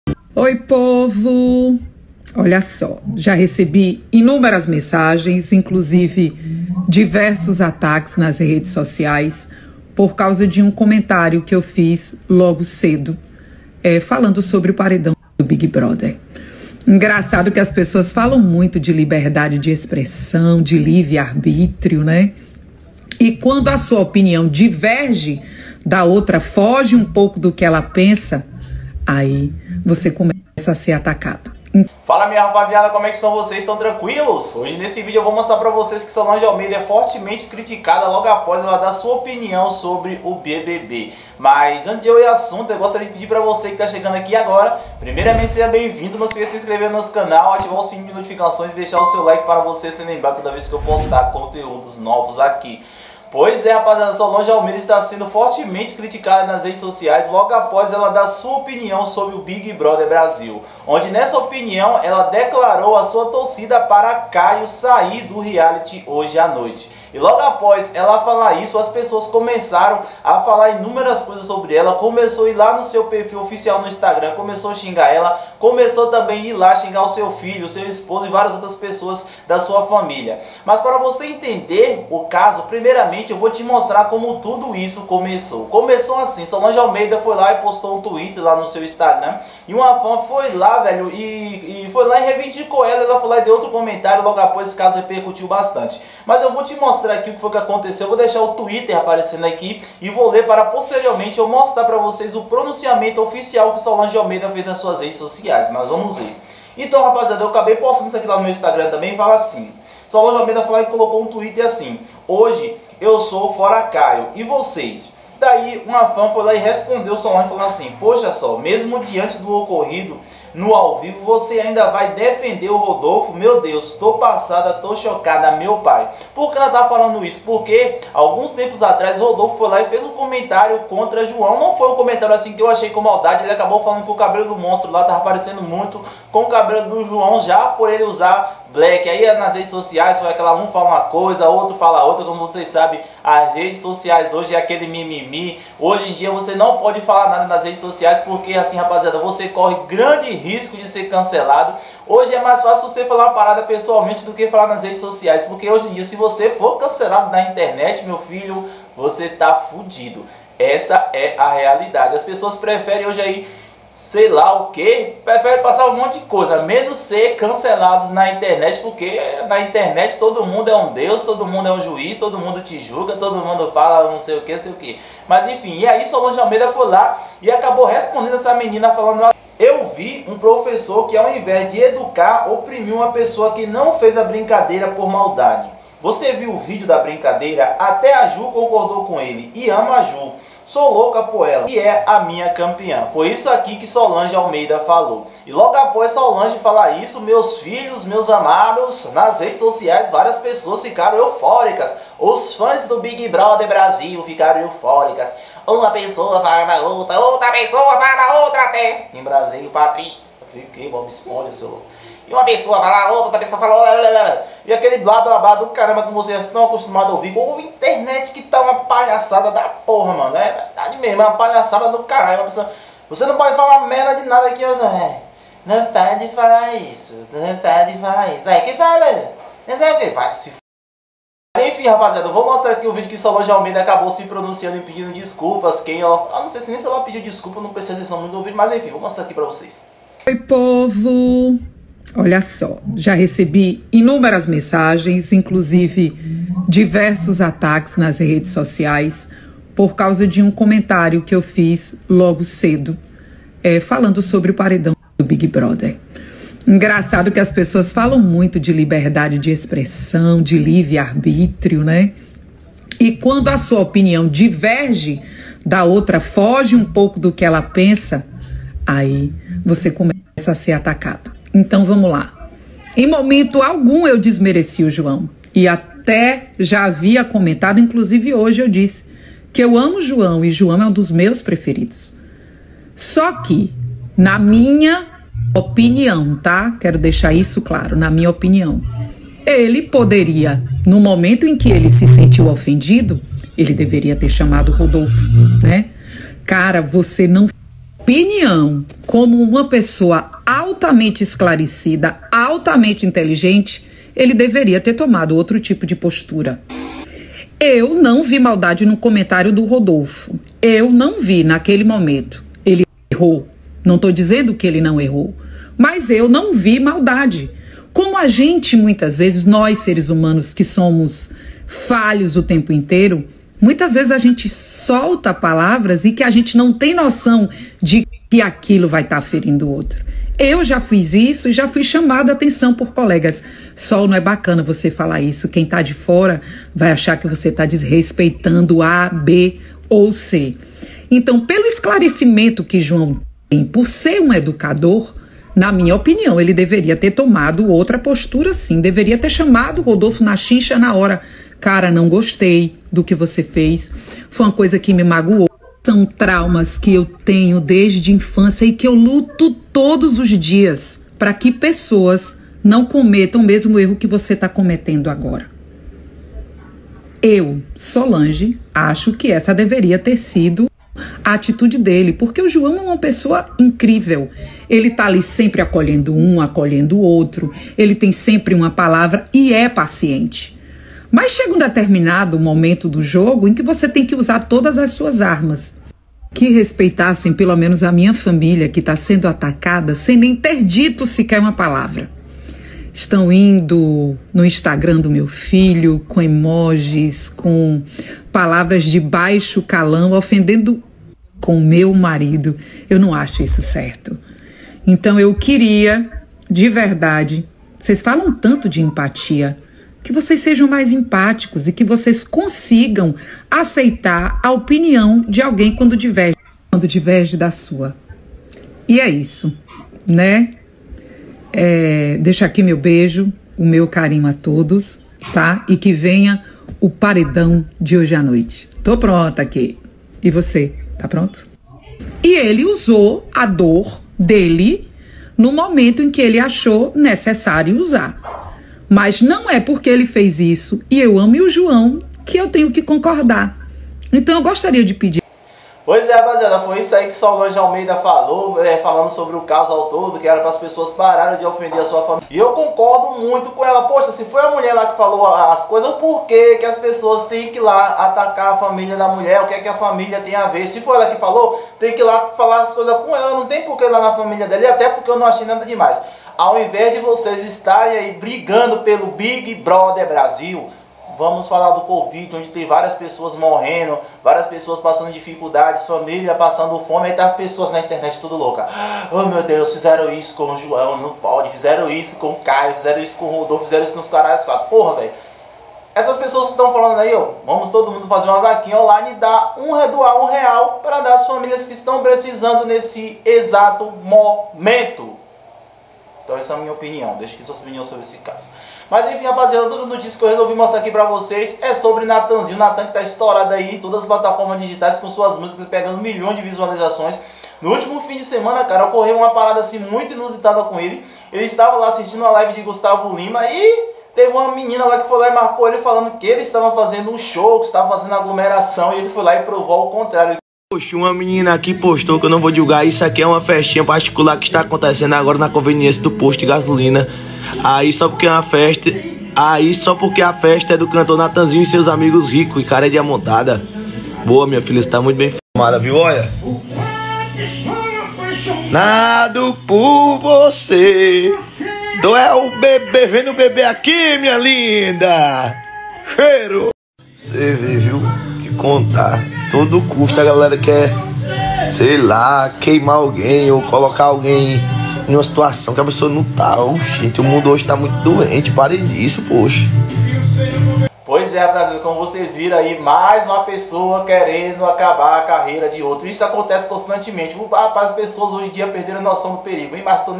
SOLANGE ALMEIDA FAZ GRANDE DESABAFO LOGO APÓS SER CANCELADA -FIZERAM SACANAGEM COM NATTAN
SOLANGE ALMEIDA FORTEMENTE CRITICADA LOGO APÓS FAZER COMENTARIO SOBRE O BBB ,E NATTAN  E VITMA DE FAKE NEWS